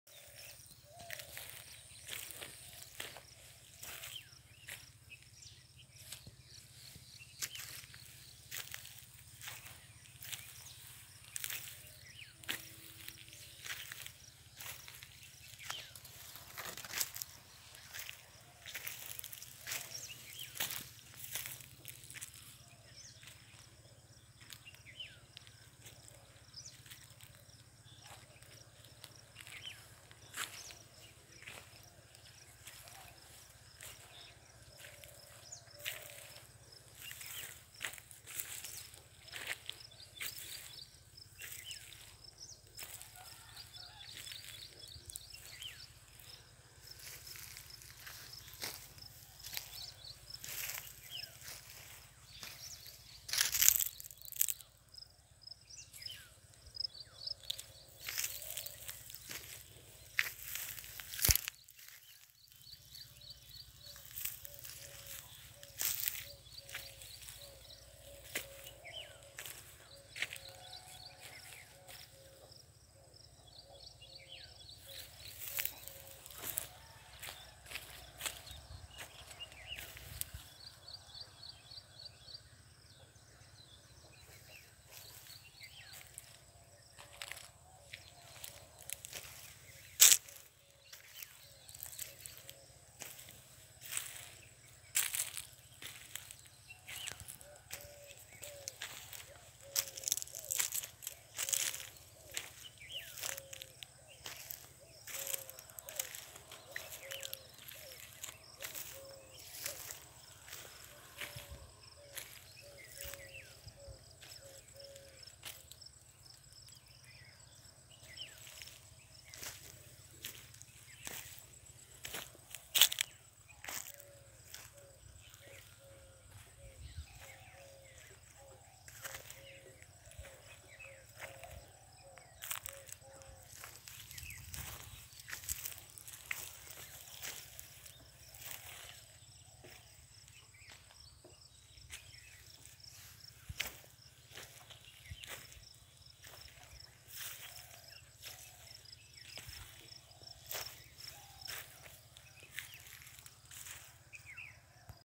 Organic Farming (অ‍র্গানিক চাষাবাদ) in west bengal ( bankura)
(chapter 1):
1. vegetable
2. orchard
3. fish ponds